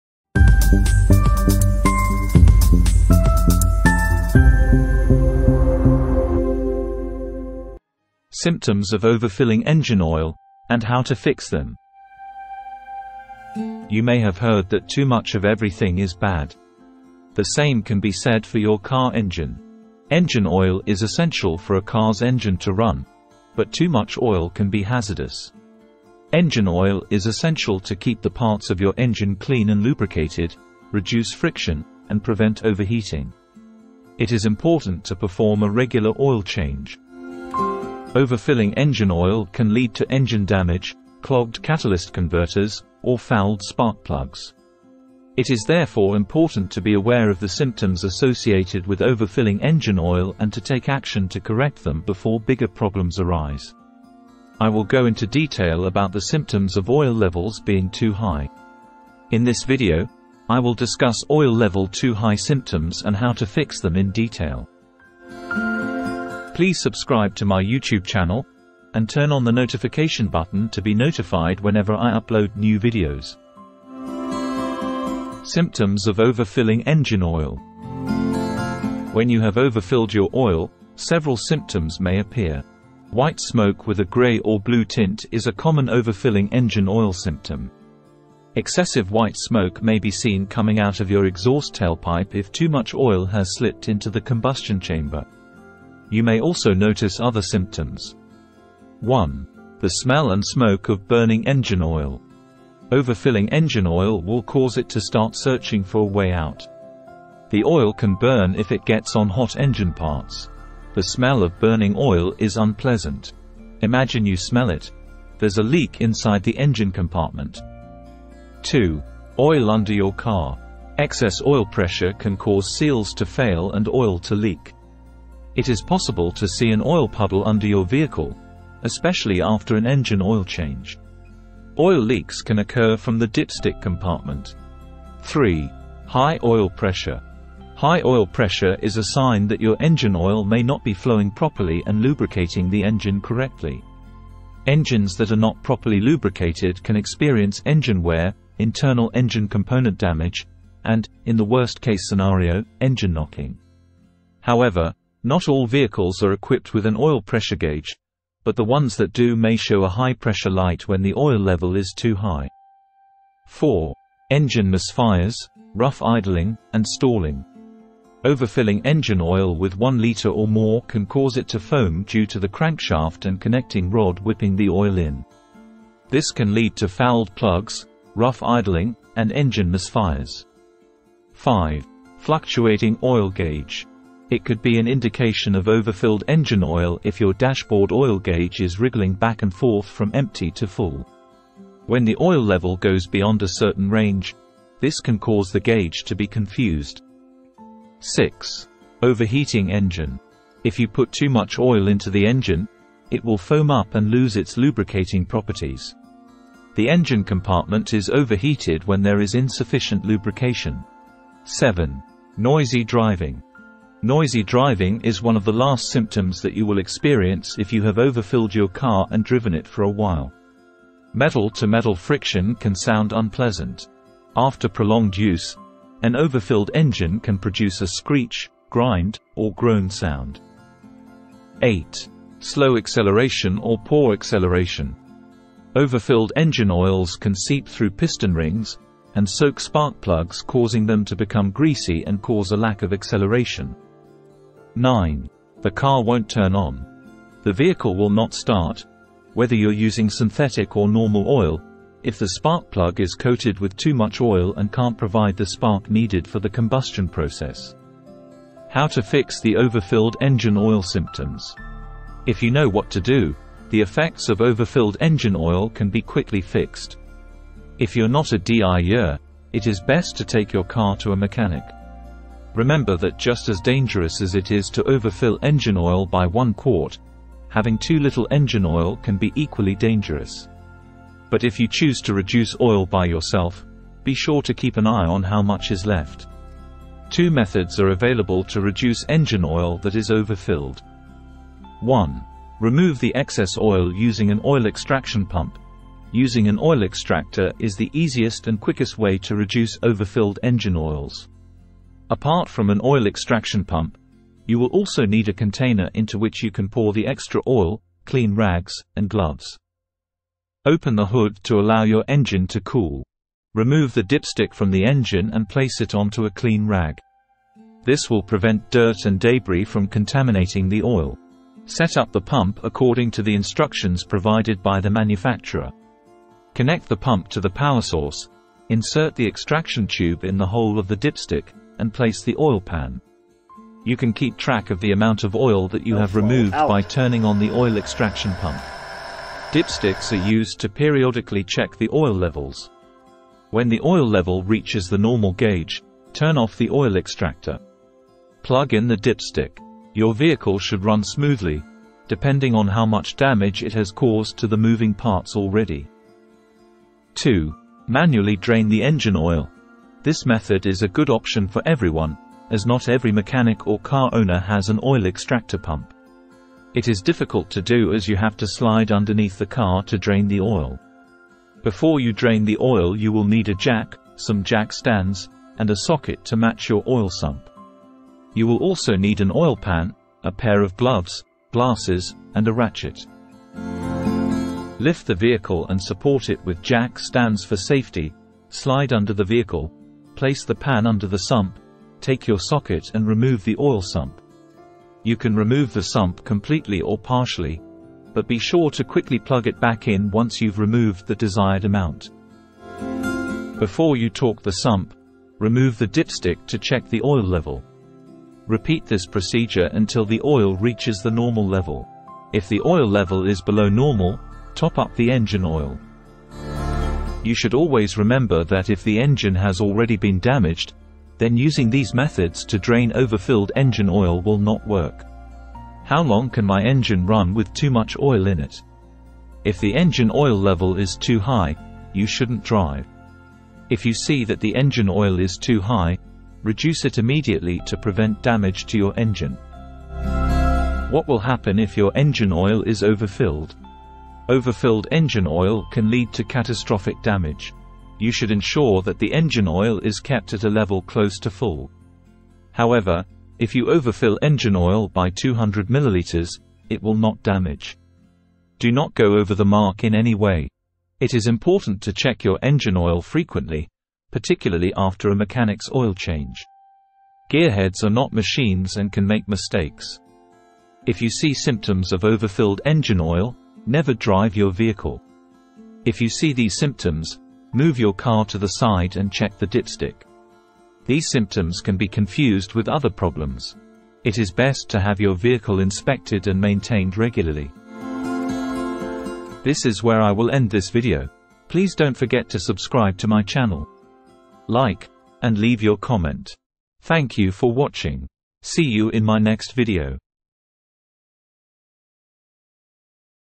Fixing Overfilled Engine Oil: Recognize Symptoms And Solutions!
Symptoms of Overfilling Engine Oil: How to Fix it
You may have heard that too much of something is bad. The same can be said for your car engine. Engine oil is essential for a car's engine to run, but too much oil can be hazardous. Engine oil is essential to keep the parts of your engine clean and lubricated, reduce friction, and prevent overheating. It is important to perform a regular oil change.
Overfilling engine oil can lead to engine damage, clogged catalyst converters, or fouled spark plugs. It is therefore important to be aware of the symptoms associated with overfilling engine oil and to take action to correct them before bigger problems arise. I will go into detail about the symptoms of oil levels being too high. In this video, I will discuss oil level too high symptoms and how to fix them in detail.
Symptoms of Overfilling Engine Oil
When you have overfilled your oil, several symptoms may appear. White smoke with a gray or blue tint is a common overfilling engine oil symptom. Excessive white smoke may be seen coming out of your exhaust tailpipe if too much oil has slipped into the combustion chamber. You may also notice other symptoms.
1. The smell and smoke of burning engine oil
Overfilling engine oil will cause it to start searching for a way out. The oil can burn if it gets on hot engine parts. The smell of burning oil is unpleasant. Imagine you smell it, there's a leak inside the engine compartment.
2. Oil under your car
Excess oil pressure can cause seals to fail and oil to leak. It is possible to see an oil puddle under your vehicle, especially after an engine oil change. Oil leaks can occur from the dipstick compartment.
3. High Oil Pressure
High oil pressure is a sign that your engine oil may not be flowing properly and lubricating the engine correctly. Engines that are not properly lubricated can experience engine wear, internal engine component damage, and, in the worst-case scenario, engine knocking. However, Not all vehicles are equipped with an oil pressure gauge, but the ones that do may show a high-pressure light when the oil level is too high.
4. Engine misfires, rough idling, and stalling
Overfilling engine oil with 1 liter or more can cause it to foam due to the crankshaft and connecting rod whipping the oil in. This can lead to fouled plugs, rough idling, and engine misfires.
5. Fluctuating Oil gauge
It could be an indication of overfilled engine oil if your dashboard oil gauge is wriggling back and forth from empty to full. When the oil level goes beyond a certain range, this can cause the gauge to be confused.
6. Overheating Engine
If you put too much oil into the engine, it will foam up and lose its lubricating properties. The engine compartment is overheated when there is insufficient lubrication.
7. Noisy Driving
Noisy driving is one of the last symptoms that you will experience if you have overfilled your car and driven it for a while. Metal-to-metal friction can sound unpleasant. After prolonged use, an overfilled engine can produce a screech, grind, or groan sound.
8. Slow acceleration or poor acceleration
Overfilled engine oils can seep through piston rings, and soak spark plugs causing them to become greasy and cause a lack of acceleration.
9. The car won't turn on
The vehicle will not start, whether you're using synthetic or normal oil, if the spark plug is coated with too much oil and can't provide the spark needed for the combustion process.
How to fix the overfilled engine oil symptoms
If you know what to do, the effects of overfilled engine oil can be quickly fixed. If you're not a DIYer, it is best to take your car to a mechanic. Remember that just as dangerous as it is to overfill engine oil by one quart, having too little engine oil can be equally dangerous. BUT If you choose to reduce oil by yourself, be sure to keep an eye on how much is left. Two methods are available to reduce engine oil that is overfilled:
Remove the excess oil using an oil extraction pump: Using an oil extractor is the easiest and quickest way to reduce Overfilled engine oils. Apart from an oil extraction pump, you will also need a container into which you can pour the extra oil, clean rags, and gloves.
Open the hood to allow your engine to cool. Remove the dipstick from the engine and place it onto a clean rag. This will prevent dirt and debris from contaminating the oil. Set up the pump according to the instructions provided by the manufacturer. Connect the pump to the power source, insert the extraction tube in the hole of the dipstick, and place the oil pan. You can keep track of the amount of oil that you have removed by turning on the oil extraction pump. Dipsticks are used to periodically check the oil levels.
When the oil level reaches the normal gauge, turn off the oil extractor. Plug in the dipstick. Your vehicle should run smoothly, depending on how much damage i